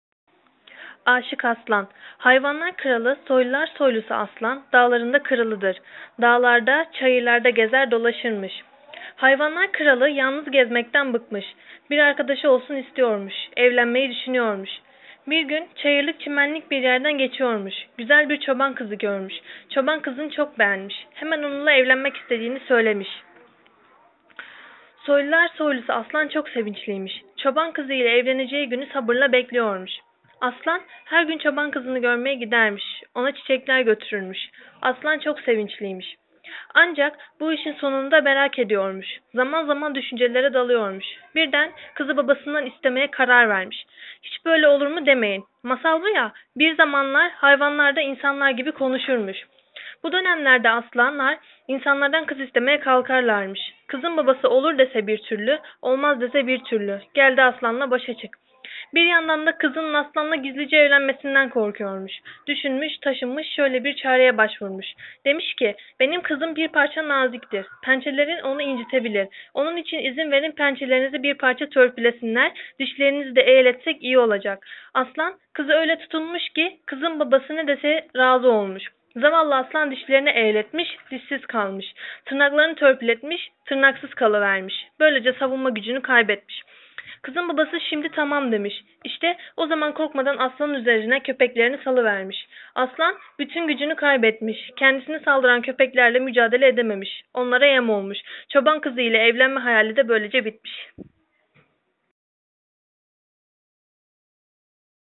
Children'Soul - Aşık Aslan
The Turkish story "Aşık Aslan" has been recorded by Turkish participants in "Children'Soul Youth Exchange Project" in 17 August 2014. The aim is to reach the children all over the world and make them know about these kind of children story.

Children'Soul Gençlik Değişim Projesi kapsamında Türk katılımcılar tarafından kaydedilen videoların amacı, başta görme engelli arkadaşlarımız olmak üzere ülkemizdeki ve diğer ülkelerdeki çocuklara ulaşmak ve hikayelerimizi dinletmektir.